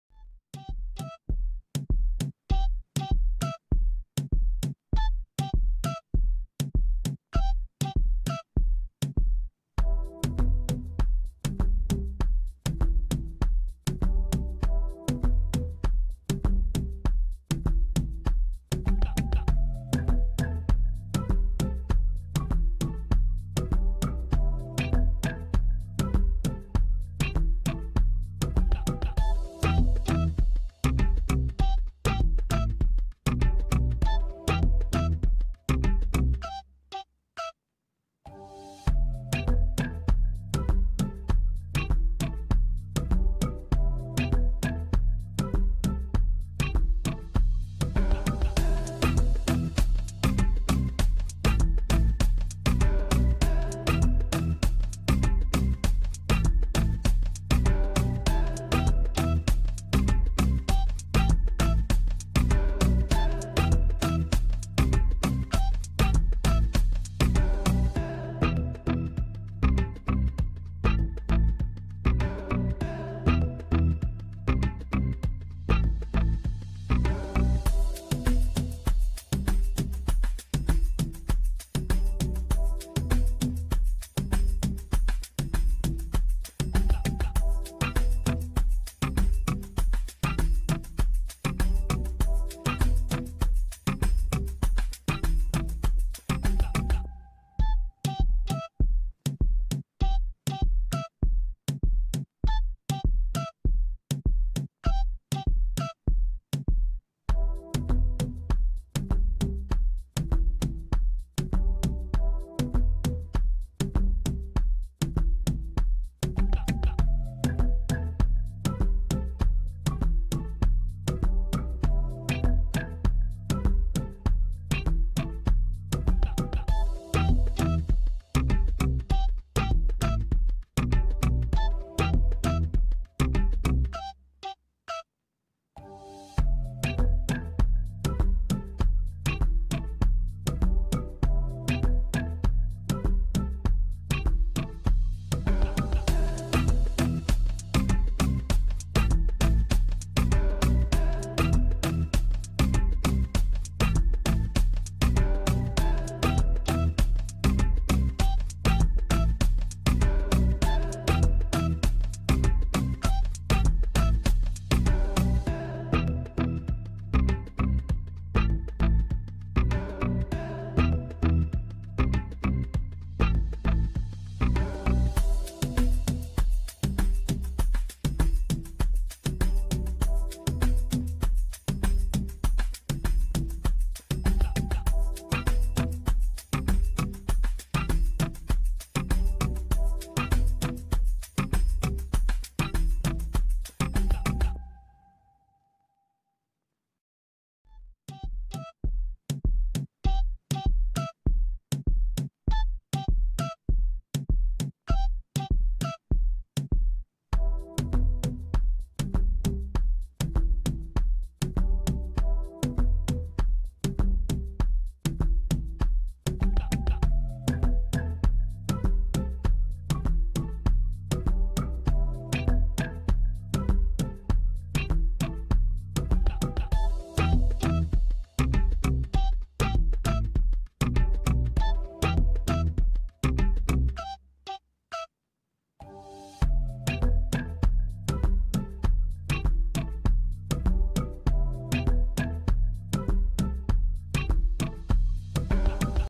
KOLAY KELEBEK PASTA YAPIMI
1 adet pandispanya
1 paket krem şanti
1 paket pudıng
2 veya 3 paket bonıbon ıstege baglı
1 adet barbıe bebek
genel izleyici için uygundur
Unutma
Cimcimeleroyunda insagram
Cimcimeleroyunda Facebook
Cimcimeler oyunda youtube